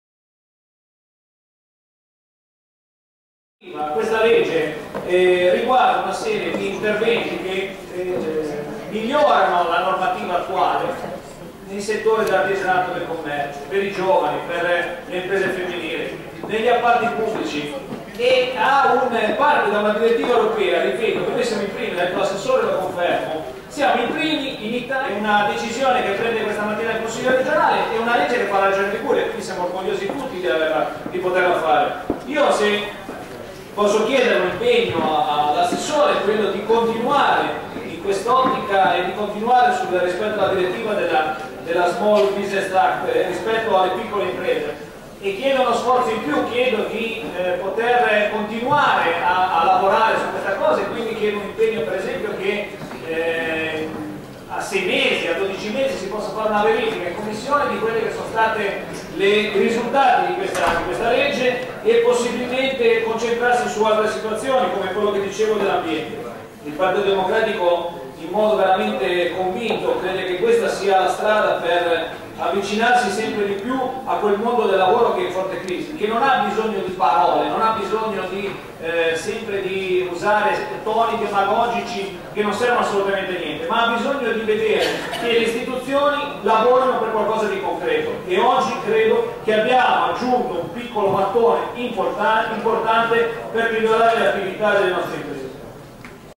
Norme per il sostegno e lo sviluppo delle micro e piccole medie imprese
Norme per il sostegno e lo sviluppo delle micro e  piccole medie imprese della Liguria Disegno di Legge 189